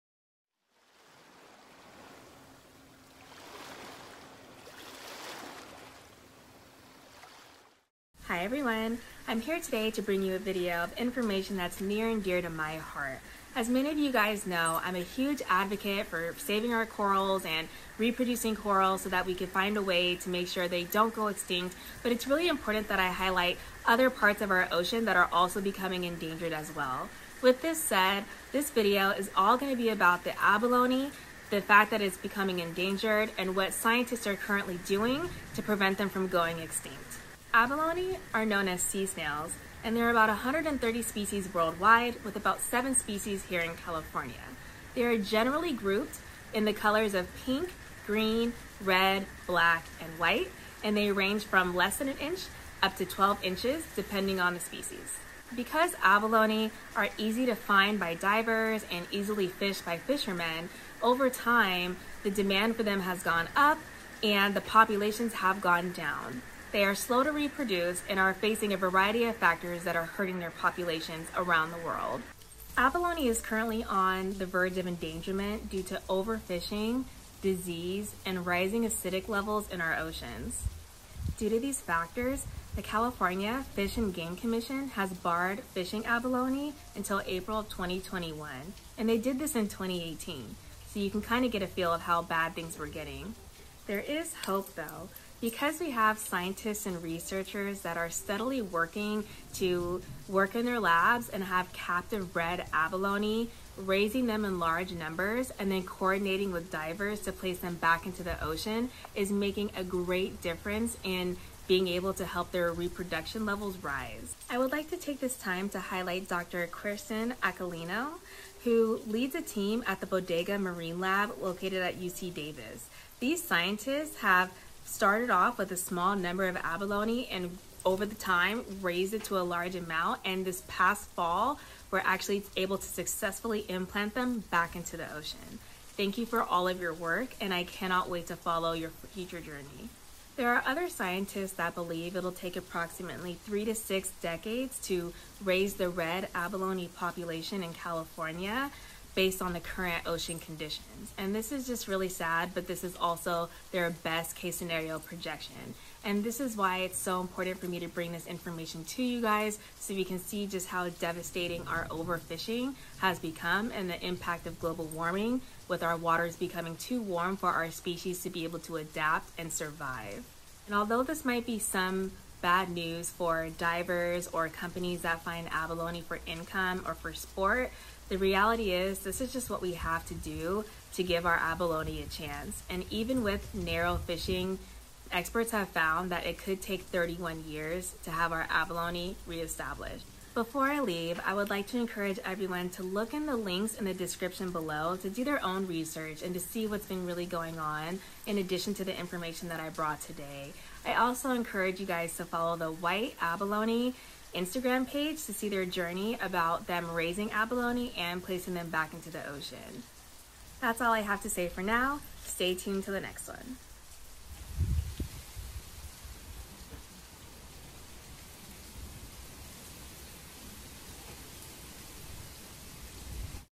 Abalone! Facts, Endangerment, & The Future
Feel free to show support!

This is a video to shed some light on what is going on with Abalone! Feel free to check out the links below, and the Bodega Marine Lab's instagram at @White_Abalone